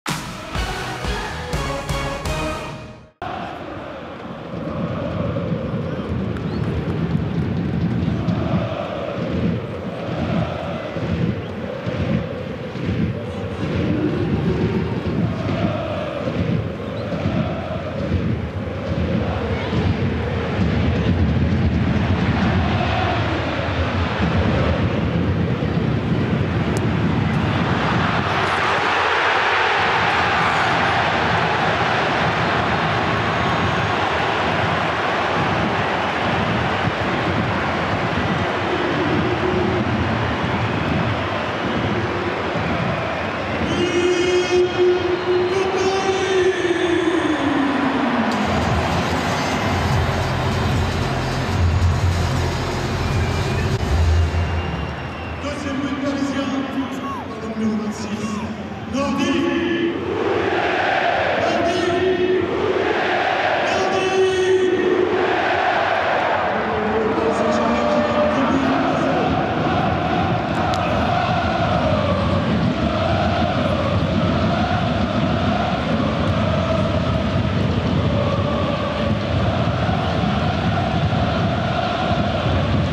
But Jaouen HADJAM (17' csc - PSG) PARIS SAINT-GERMAIN - FC NANTES (4-2) 22/23
"PARIS SAINT-GERMAIN - FC NANTES (4-2) But de Jaouen HADJAM (17' csc - PSG)  + Tous les buts de PARIS SAINT-GERMAIN - FC NANTES en vidéo.

Ligue 1 Uber Eats - Saison 2022/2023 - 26ème journée PARC DES PRINCES - samedi 4 mars 2023

Buteurs : Lionel Andres MESSI CUCCITTINI (12' - PSG) / Jaouen HADJAM (17' csc - PSG) / Ludovic BLAS (31' - FCN) / Ignatius GANAGO (38' - FCN) / Danilo Luis Helio PEREIRA (60' - PSG) / Kylian MBAPPE (90' +2 - PSG)